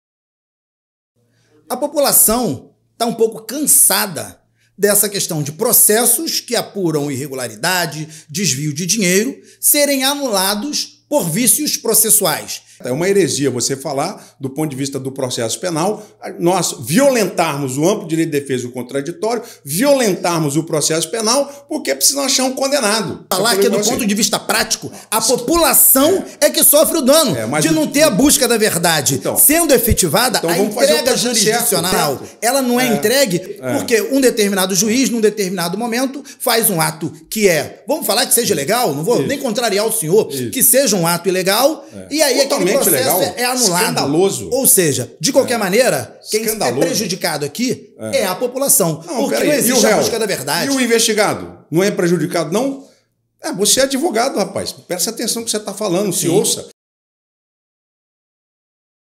TRETA: WITZEL X DR. ZEMA NO PODCAST
Pela primeira vez em um podcast, o EX-GOVERNADOR WILSON WITZEL enfrenta DURAS VERDADES com DR.ZEMA

Instagram Dr.Zema : @drzemaoficial
Instagram Wilson Witzel : @wilsonwitzel

ANFITRIÃO

Fábio Gideão - Facebook / Instagram: @fabiogideao

SITE: PortaldoGida.com ______________________________________
QUEM AMA PROTEGE ❤
Já são + de 50 mil pets identificados 🥰
🐶 Estamos construindo a maior comunidade de identificação e cuidados pet do Brasil.